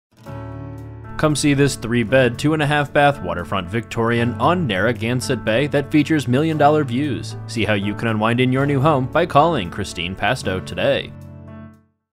2 Narragansett Avenue Riverside, RI | ColdwellBankerHomes.com
2 Narragansett Avenue, Riverside, RI

Come see this waterfront Victorian on Narragansett Bay with million-dollar views.  Enjoy Westerly sunsets from the new wood and wire deck.  Look west to RI Yacht Club and north to the lighthouse!!!  A bright foyer welcomes guests to the large L-shaped living room that has a separate sitting area and